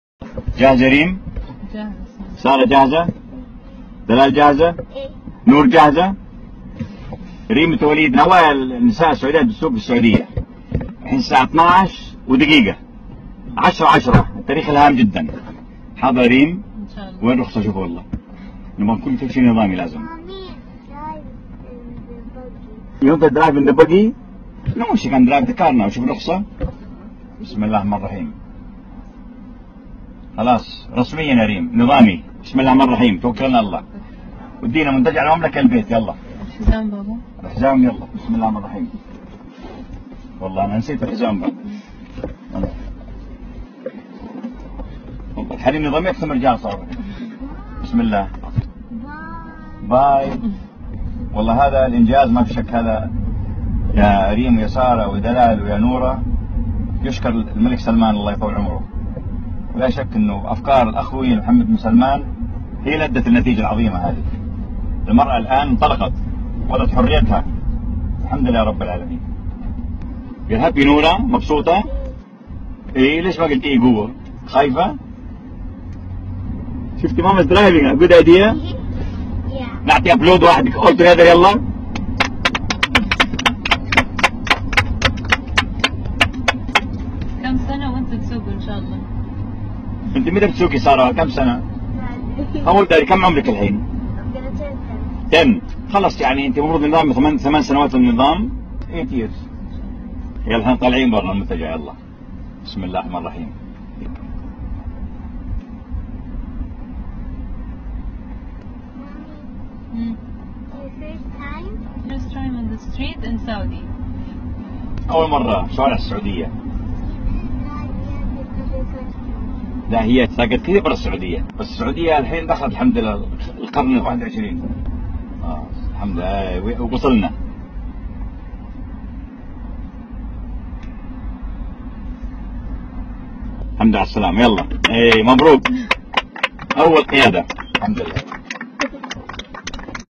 شاهد الأميرة ريم بنت الوليد بن طلال تنفذ قرار "قيادة المرأة" فى أولى لحظات تطبيقه
سجّلت الأميرة ريم بنت الوليد بن طلال، نفسها كأول سعودية تقود السيارة، مع أول دقيقة لبدء تنفيذ قرار قيادة المرأة، اليوم الأحد، في تمام الساعة الثانية عشرة ودقيقة واحدة صباحًا، في العاصمة الرياض.

ونشر الأمير الوليد فيديو لابنته ريم وهي تقود سيارتها، عبر حسابه بموقع التواصل الاجتماعي "تويتر"، وقال في الفيديو: في وقت مبكّر من صباح اليوم الاحد "الآن الساعة 12:01 من صباح يوم عشرة شوال تاريخ مهم جدًّا (24 يونيو 2018) مع ابنتي ريم وهي تسوق بي وحفيداتي في الرياض".

وأضاف الوليد: "كفى نقاشًا.. حان وقت قيادة المرأة للسيارة".

وقال: "نشكر الملك سلمان.. المرأة الآن انطلقت".

وتبدأ المرأة السعودية قيادة السيارات اليوم الأحد 24 يونيو، تنفيذًا لقرار اتّخذه خادم الحرمين الشريفين الملك سلمان بن عبدالعزيز العام الماضي، يسمح للمرأة بقيادة السيارة، وإصدار الرخص لها.